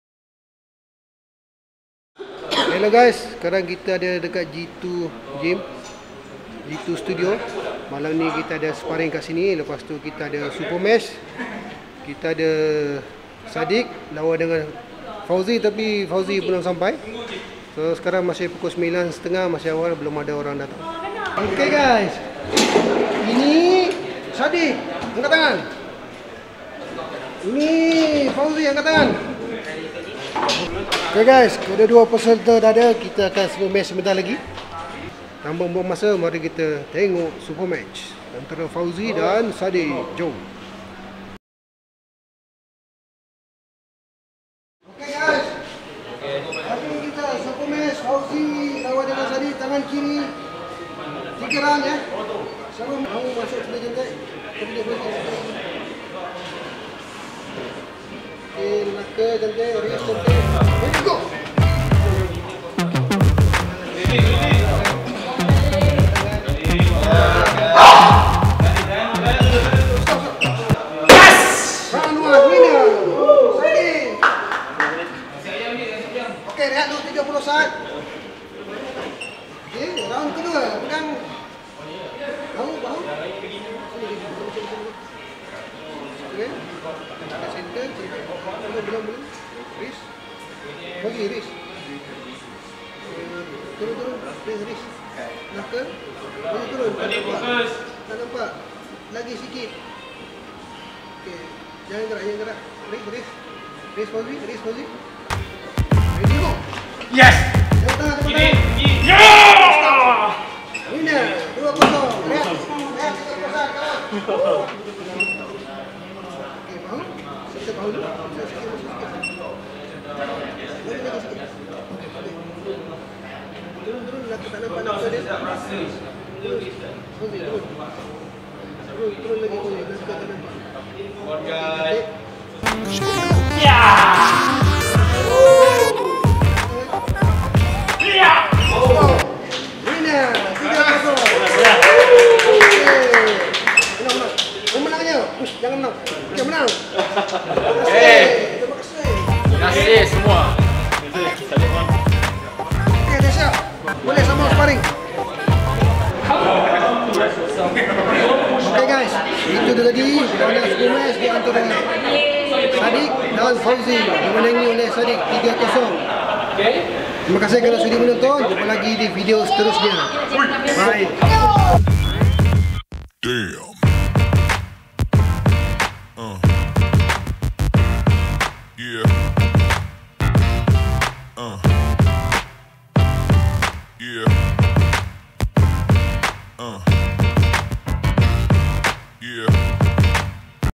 Supermatch Gusti Lengan Fauzi Sepang vs Sadiq Cheras
Supermatch gusti lengan di antara Fauzi (Champ Sepang) & Sadiq (Champ Cheras) lawan tangan kiri 3 pusingan di G2 Studio & Fitness Kota Damansara.

Armwrestling Federation
Malaysia Armwrestling Federation

Putrajaya Armwrestling
Negeri Sembilan Armwrestling
Kedah Armwrestling
Perak Armwrestling
Perlis Armwrestling